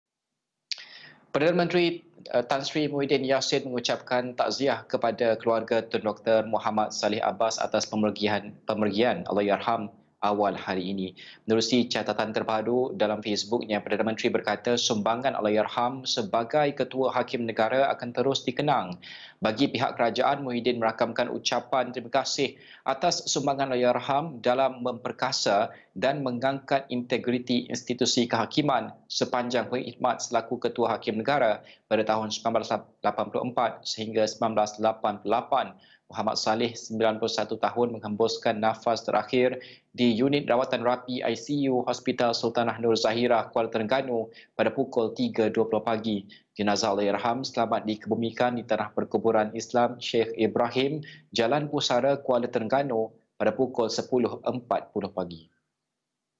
Nasional | Perdana Menteri ucap takziah kepada keluarga Tun Dr Mohamed Salleh Abas
Perdana Menteri Tan Sri Muhyiddin Yassin mengucapkan takziah kepada keluarga Tun Dr Mohamed Salleh Abas atas pemergian Allahyarham, awal hari ini.
Menerusi catatan terbaharu dalam Facebooknya, Perdana Menteri berkata, sumbangan Allahyarham sebagai Ketua Hakim Negara akan terus dikenang.